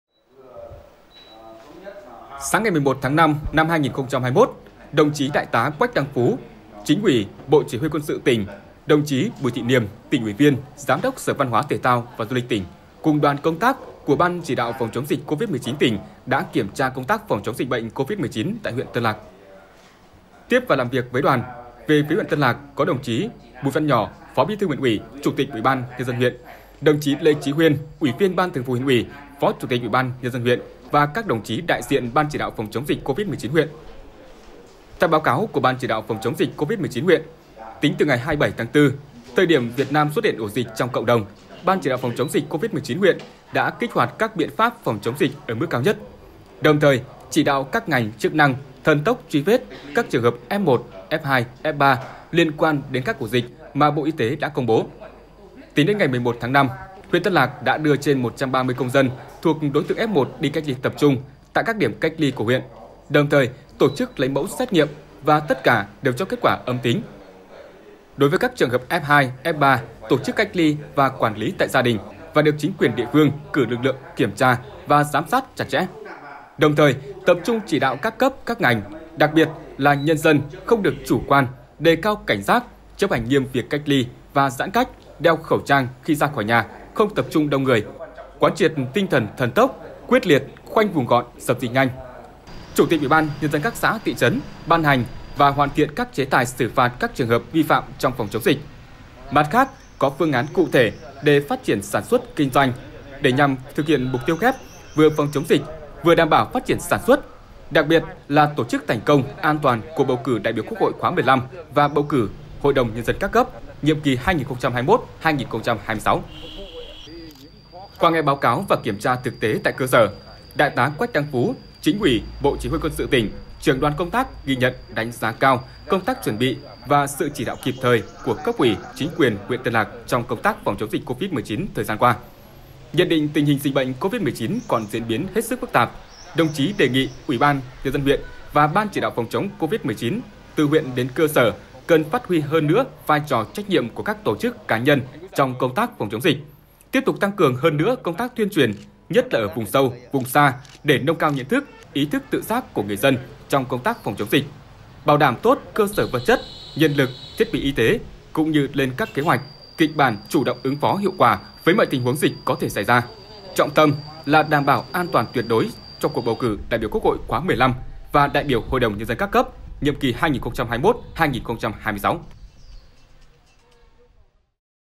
BCĐ Phòng chống dịch Covid-19 tỉnh HB kiểm tra công tác phòng chống dịch Covid-19 tại huyện Tân Lạc
Ban Chỉ đạo Phòng chống dịch Covid-19 tỉnh Hòa Bình kiểm tra công tác phòng chống dịch Covid-19 tại huyện Tân Lạc